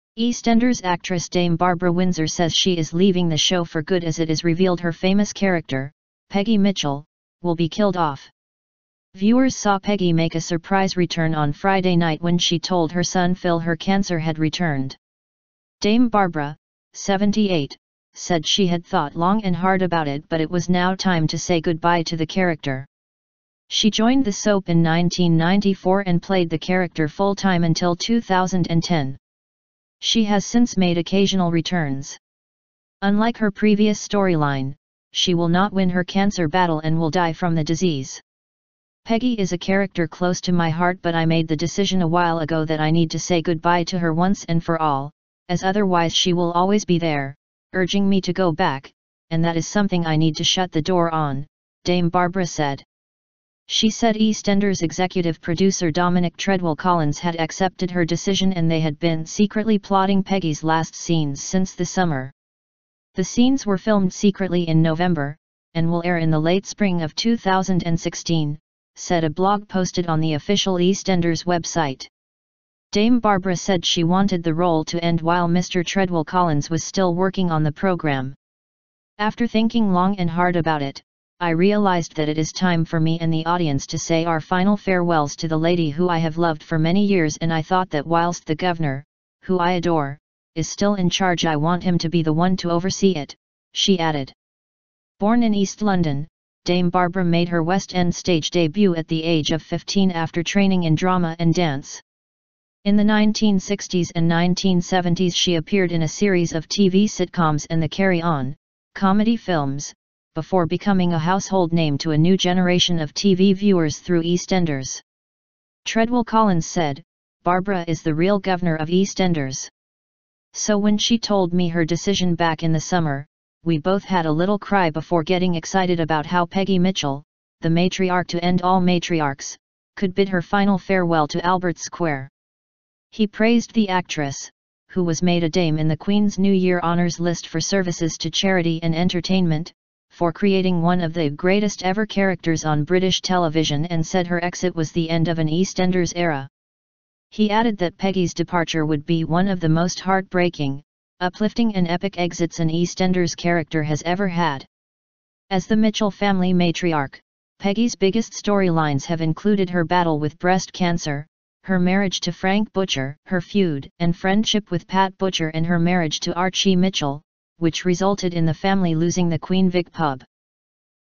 Dame Barbara Windsor to leave EastEnders for good
EastEnders actress Dame Barbara Windsor says she is leaving the show for good as it is revealed her famous character, Peggy Mitchell, will be killed off.

Viewers saw Peggy make a surprise return on Friday night when she told her son Phil her cancer had returned.

Dame Barbara, 78, said she had thought "long and hard about it" but it was now time to say goodbye to the character.

She joined the soap in 1994 and played the character full-time until 2010. She has since made occasional returns.

Unlike her previous storyline, she will not win her cancer battle and will die from the disease.

"Peggy is a character close to my heart but I made the decision a while ago that I need to say goodbye to her once and for all, as otherwise she will always be there, urging me to go back, and that is something I need to shut the door on," Dame Barbara said.

She said EastEnders executive producer Dominic Treadwell-Collins had accepted her decision and they had been "secretly plotting Peggy's last scenes" since the summer.

The scenes were filmed secretly in November, and will air in the late spring of 2016, said a blog posted on the official EastEnders website.

Dame Barbara said she wanted the role to end while Mr Treadwell-Collins was still working on the programme.

"After thinking long and hard about it, I realised that it is time for me and the audience to say our final farewells to the lady who I have loved for many years and I thought that whilst the guvnor, who I adore, is still in charge I want him to be the one to oversee it," she added.

Born in east London, Dame Barbara made her West End stage debut at the age of 15 after training in drama and dance. In the 1960s and 1970s she appeared in a series of TV sitcoms and the Carry On... comedy films, before becoming a household name to a new generation of TV viewers through EastEnders.

Treadwell-Collins said: "Barbara is the real guvnor of EastEnders. So when she told me her decision back in the summer, we both had a little cry before getting excited about how Peggy Mitchell, the matriarch to end all matriarchs, could bid her final farewell to Albert Square."

He praised the actress, who was made a dame in the Queen's New Year Honours list for services to charity and entertainment, for creating one of the "greatest ever characters on British television" and said her exit was the end of an EastEnders era.

He added that Peggy's departure would be "one of the most heartbreaking, uplifting and epic exits an EastEnders character has ever had".

As the Mitchell family matriarch, Peggy's biggest storylines have included her battle with breast cancer, her marriage to Frank Butcher, her feud and friendship with Pat Butcher and her marriage to Archie Mitchell, which resulted in the family losing The Queen Vic pub.